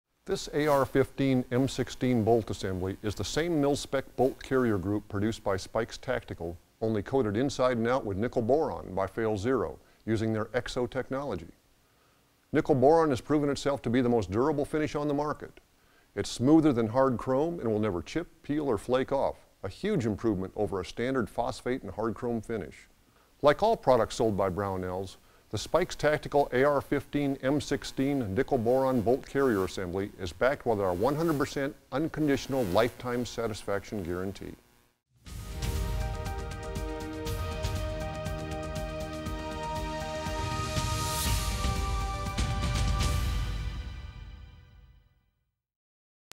Brownells - Spikes Tactical Nickle Boron Bolt Carrier Group 5.56
This video showcases Spikes Tactical Nickle Boron Bolt Group.